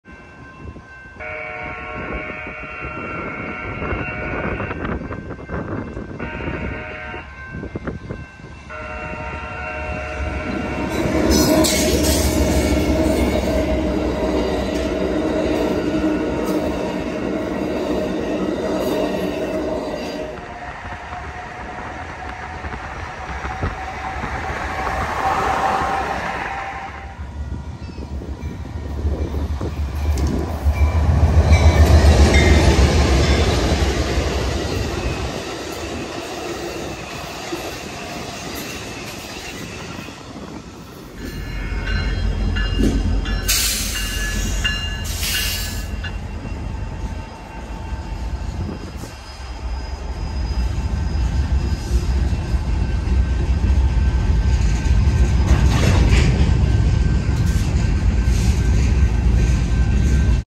Amtrak #2 With Two Special Livery Engines! 4K
We set out to catch Amtrak #2 coming into Houston and saw the train with both #184 and #130 engines. Both engines were painted in 2011 for the 40 year anniversary of Amtrak.